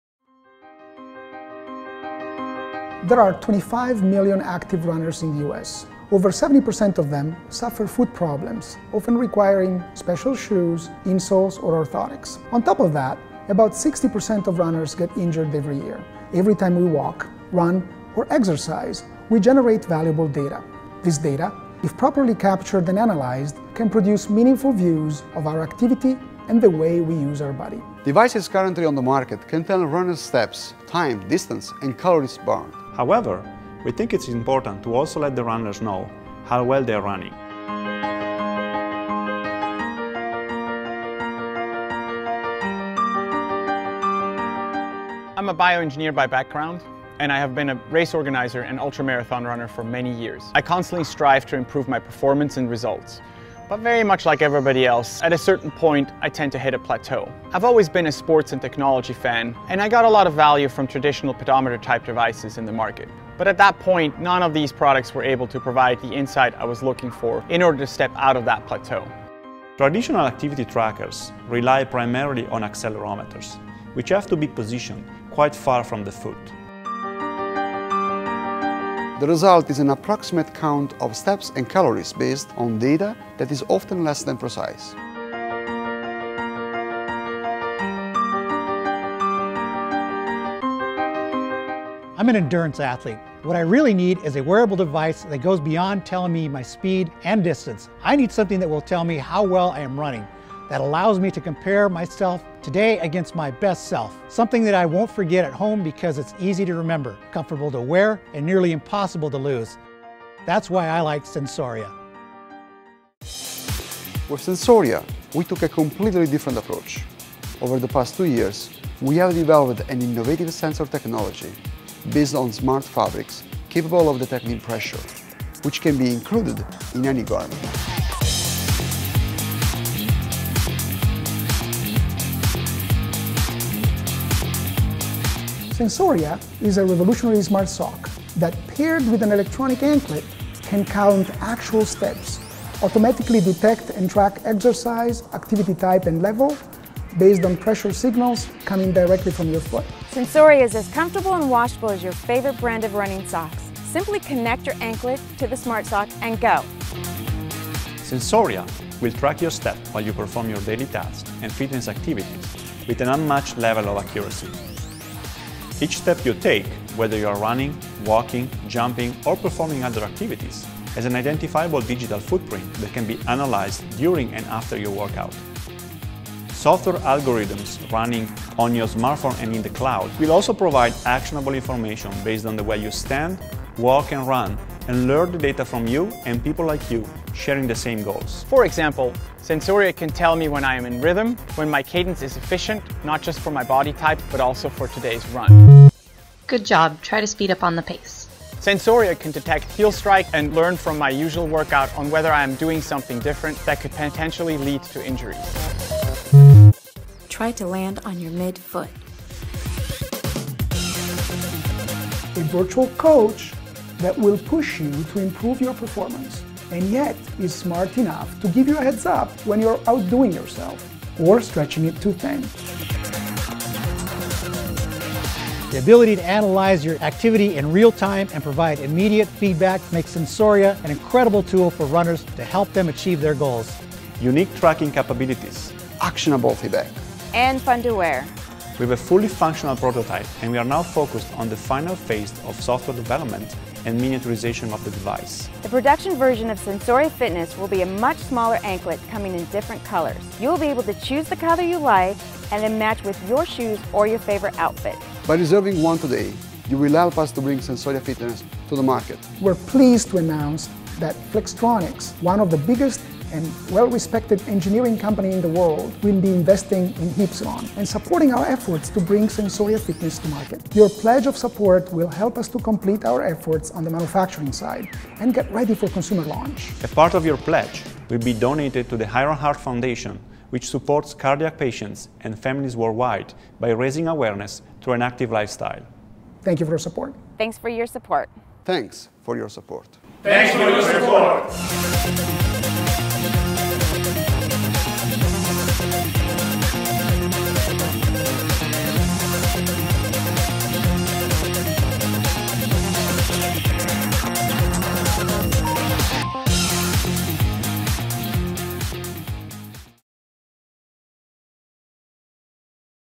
Heapsylon Launches Wearable Technology Sensoria Fitness Smart Socks for Runners
More than 60% of runners will suffer some type of injury this year but what if there was a smart sock that could alert when they are running incorrectly? To the rescue comes wearable technology Sensoria Fitness Socks for runners that not only track steps, calories, speed and distance, but also how far and fast they run, where they land on their feet, and how they stride. This informs the runner on how to reach goals and potentially prevent injuries.